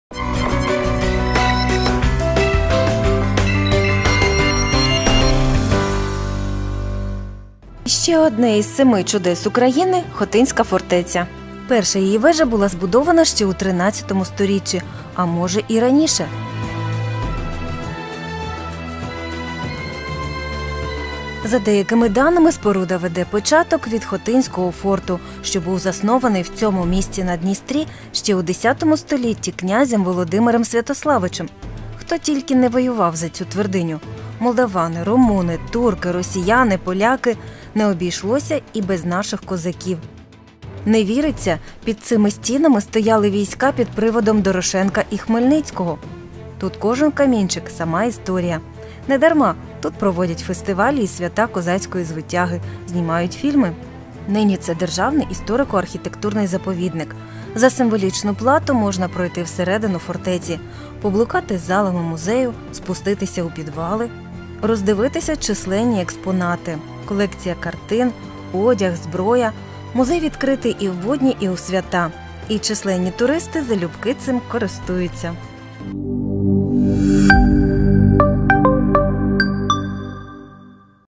07.05.2019. Новий день. Галина Щерба із Хотинської фортеці
Одне із семи чудес України – Хотинська фортеця. Перша її вежа була збудована ще у 13-му сторіччі, а може, і раніше. Тим більше, що Хотинський форт, від якого бере початок укріплення, був заснований в цьому місці на Дністрі  ще у X столітті князем Володимиром Святославичем.
Нині тут Державний  історико-архітектурний заповідник.  Музей відкритий і в будні, і у свята, і численні туристи залюбки цим користуються.